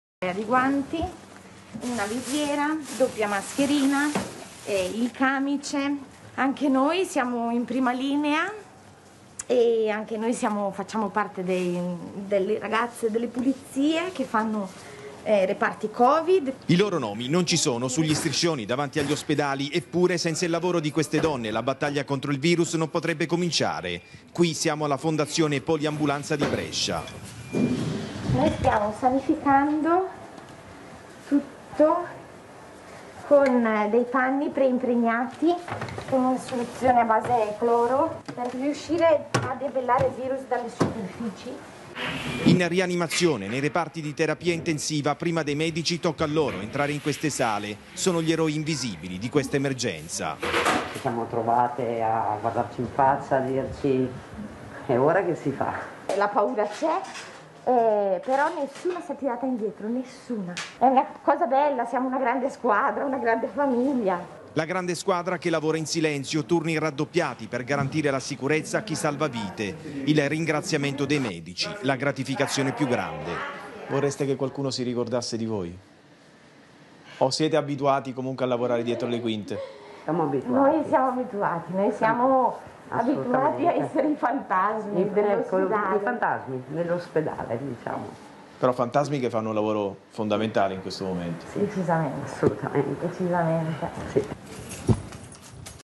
Coronavirus: il Tg1 ha intervistato gli eroi invisibili di Poliambulanza
Sono le signore addette alla sanificazione dei reparti Covid. Senza di loro la battaglia contro il coronavirus e la ripartenza verso un ospedale sicuro per tutti, non potrebbero iniziare. La loro gratificazione più grande è la riconoscenza del personale sanitario.